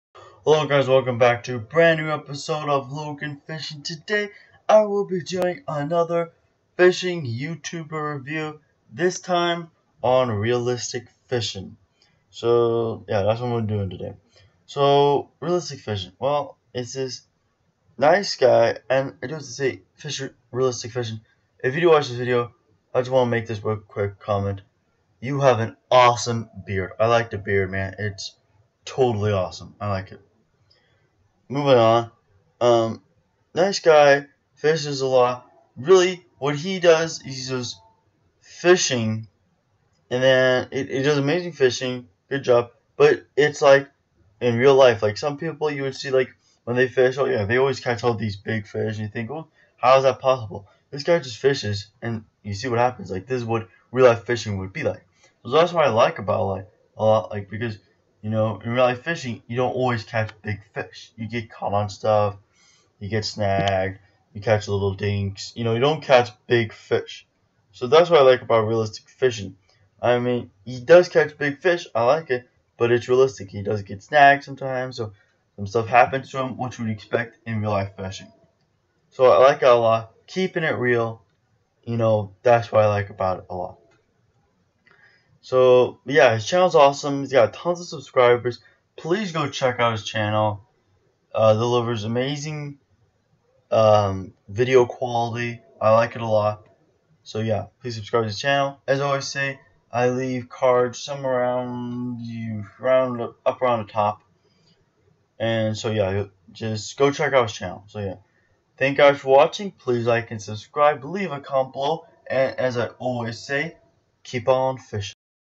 Realistic Fishing/Fishing Youtubers Review
It's another FYR(Fishing Youtubers Review) this time on Realistic Fishing. Please go check out his channel. As I always say Keep on fishin"

Hello subscribers and new comers to my channel, I'm running out of ideas for videos.